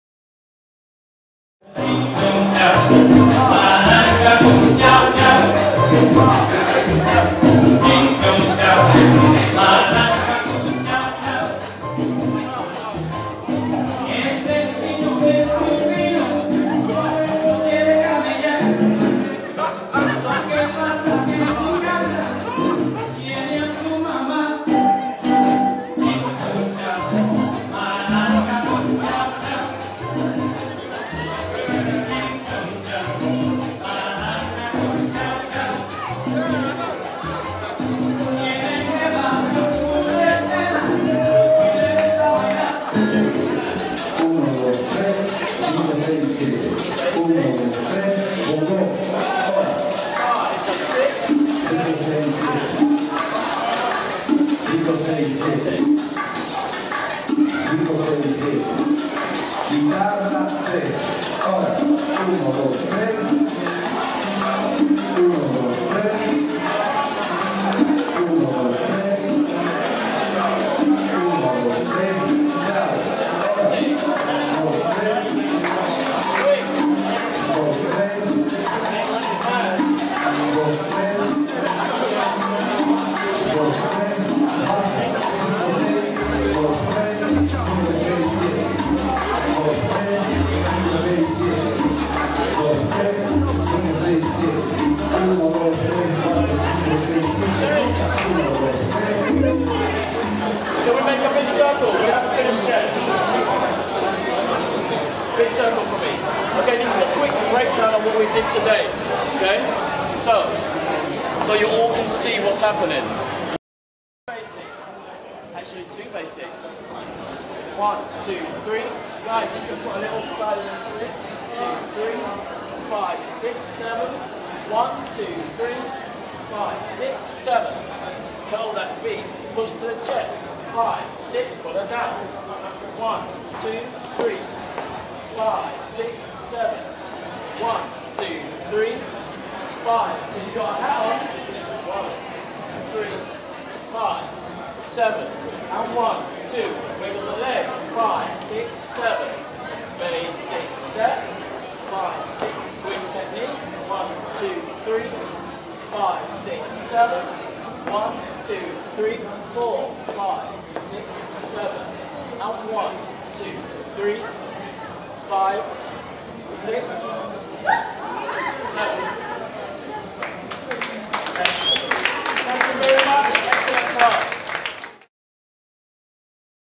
Ritmo Latino with phillip the salsa wizard
This is an advanced and very challenging class taught by Phillip Langlais aka the salsa wizard @ club 19 in trent park Oakwood,keep an eye out for the Ritmo Latino special with the salsa wizard coming soon. enjoy